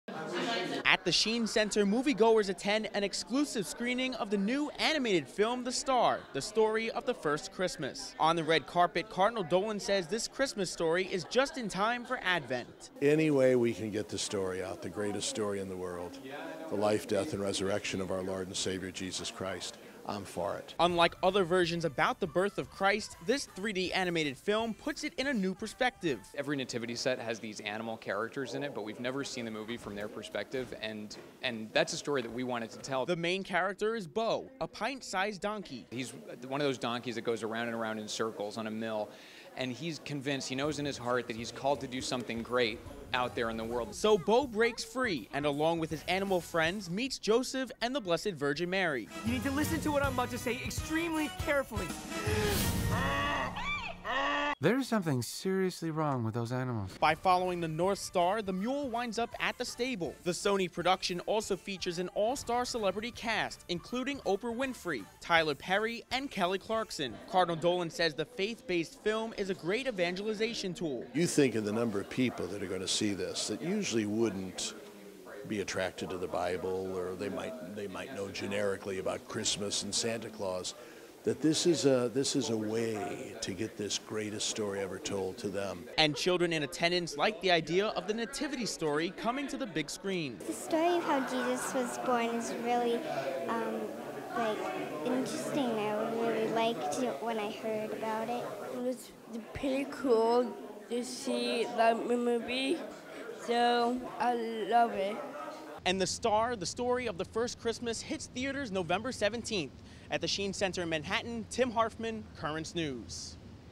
Sheen Center Screens New Christmas Movie
The story of the Nativity - the first Christmas - debuts at a gala hosted by Cardinal Timothy Dolan. The animated motion picture features an all-star cast of voices. Currents News Tim Harfmann reports from the Catholic cultural center in Manhattan.